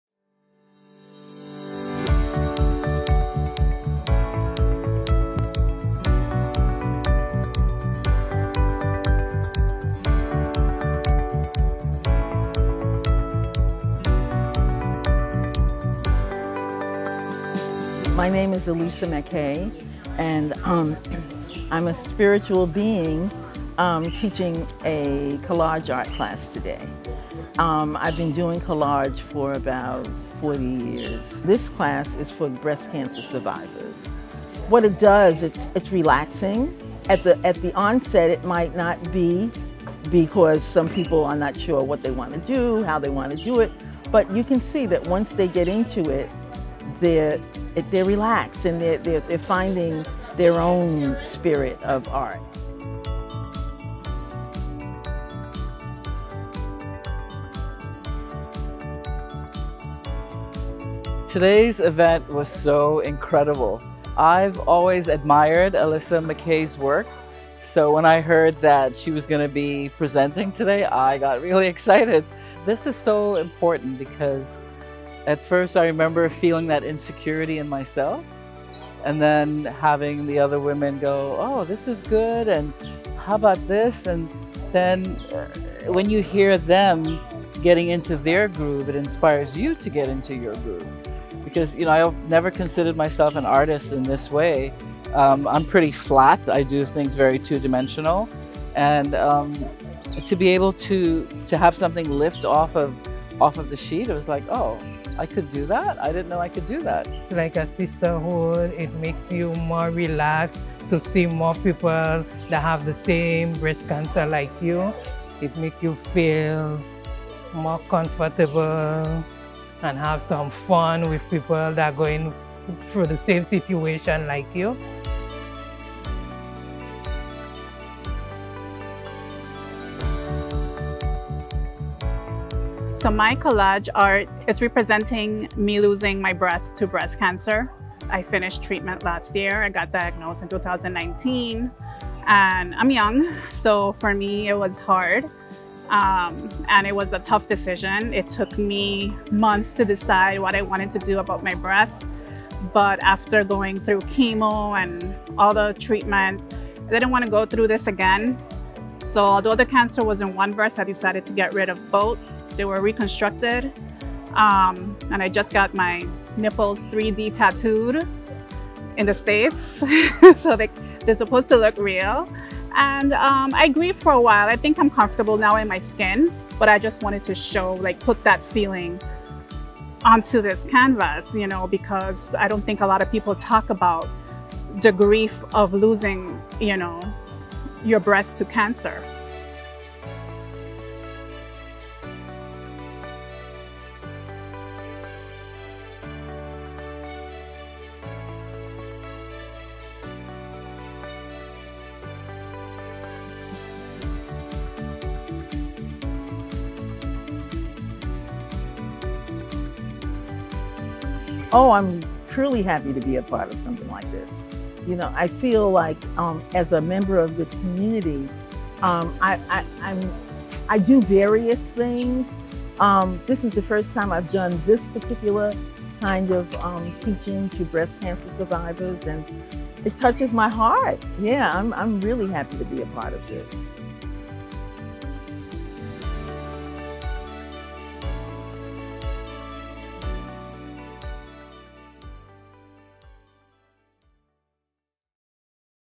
Art Therapy for Breast Cancer Survivors - Collage Art With Elisa McKAy
Breast Cancer Project Collage Art Class with Elisa McKay held at the Caribbean Museum for the Arts St.Croix US Virgin Islands.